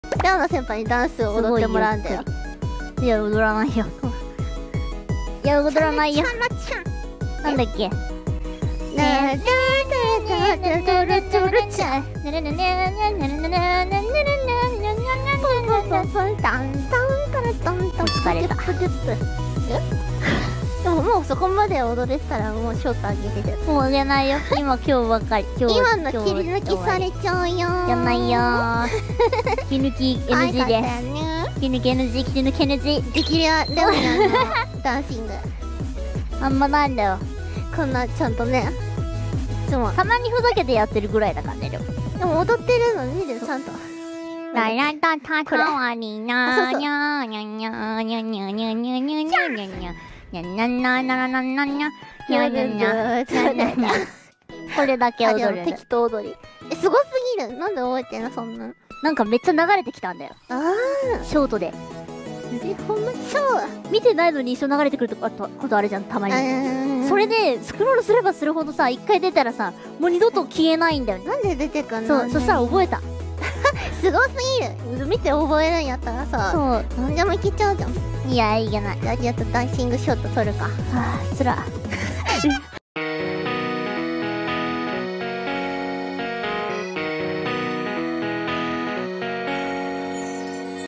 激レア！ダンシング！かわいい猫語で歌う獅子神レオナ【Re:AcT/獅子神レオナ/月紫アリア/切り抜き】
◾︎配信元
【歌枠/singing】✨全身3D✨野生の大先輩を捕獲したから、いっしょに歌うぞ💜 karaoke【月紫アリア/獅子神レオナ/ NeclioN /Vtuber】